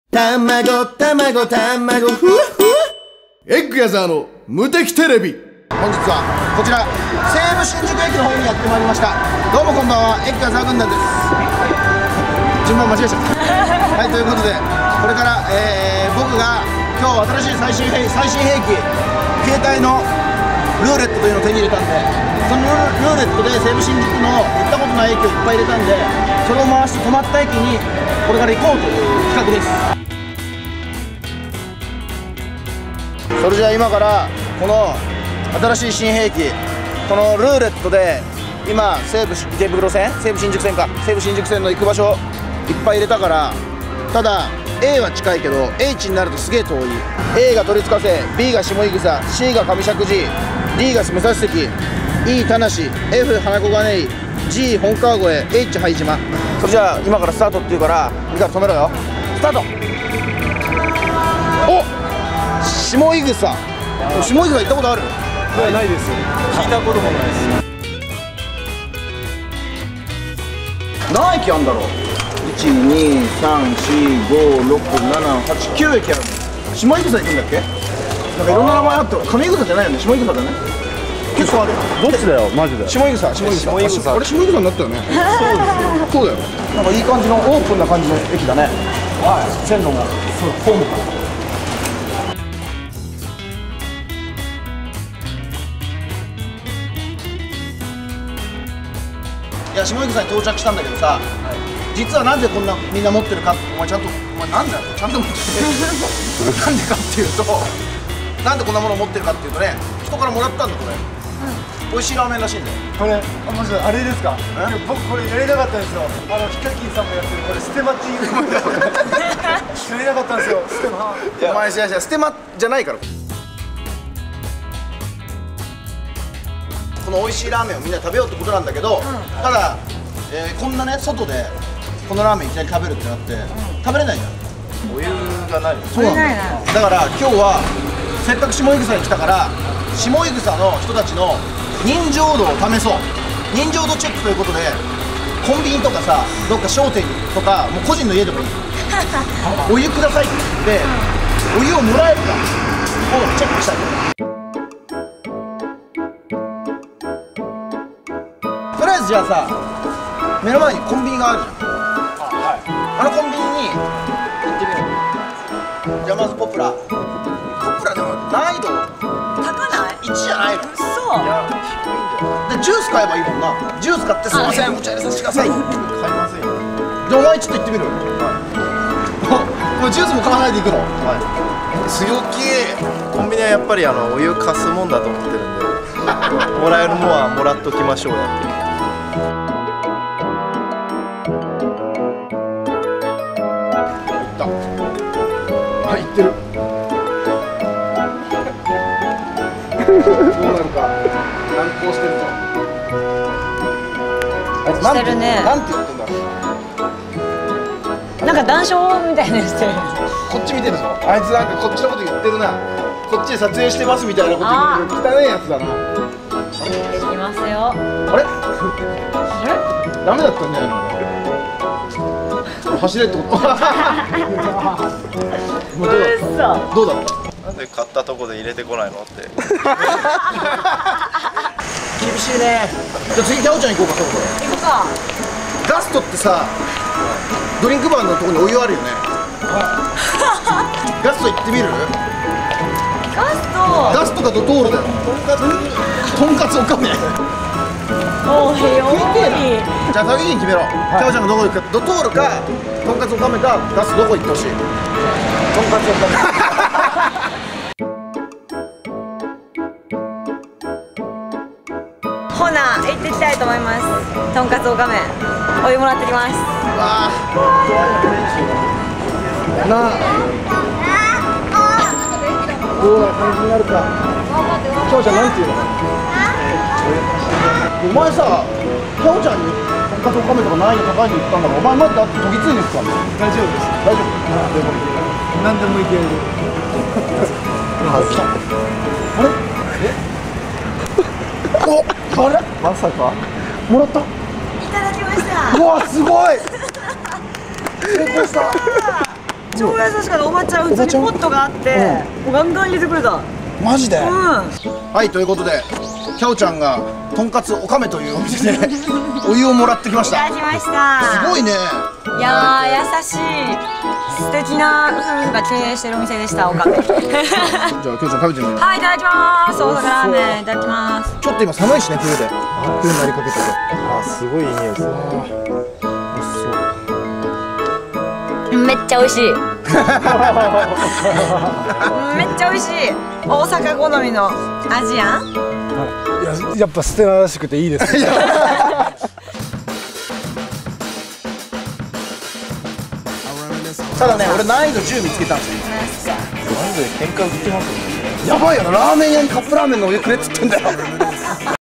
【初めてのステマ】 買ってないお店でカップ麺のお湯をもらえるか？
ウッス！今回は実験だ！
カップ麺のお湯をもらいに下井草まで行ってきたぞ！

下井草の人情度をチェックだ！
最後まで見てくれよな！

そして明日はTAKEKINが奇跡を起こすぜ！